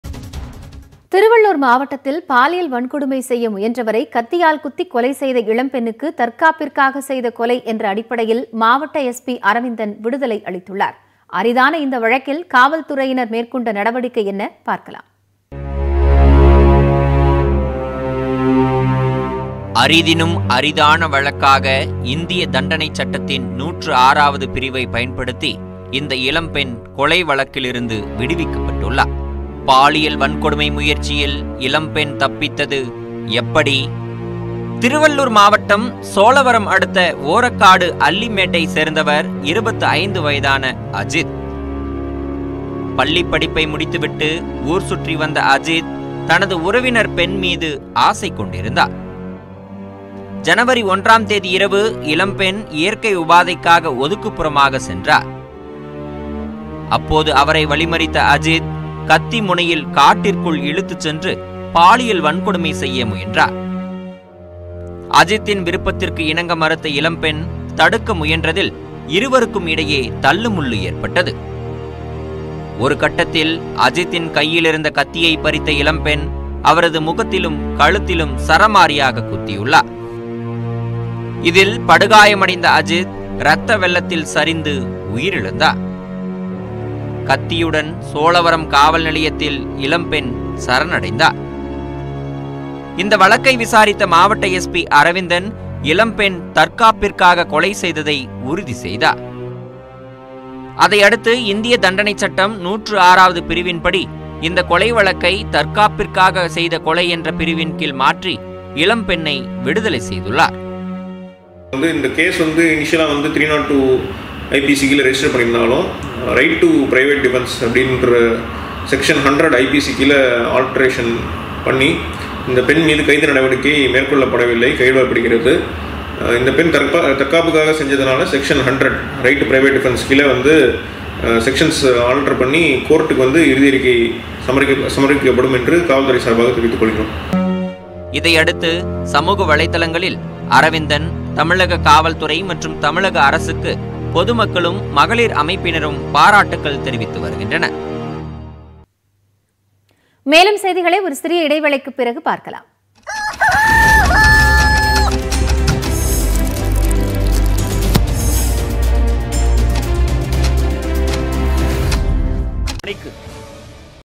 பாலியல் வன்கொடுமை செய்ய முயன்றவர் கத்தியால் குத்திக் கொலை | Tamil Crime News
பாலியல் வன்கொடுமை முயற்சி | தற்காப்புக்காக செய்த கொலை என்ற பிரிவில் இளம்பெண் விடுவிப்பு | பாலியல் வன்கொடுமை செய்ய முயன்றவர் கத்தியால் குத்திக் கொலை | Tamil Crime News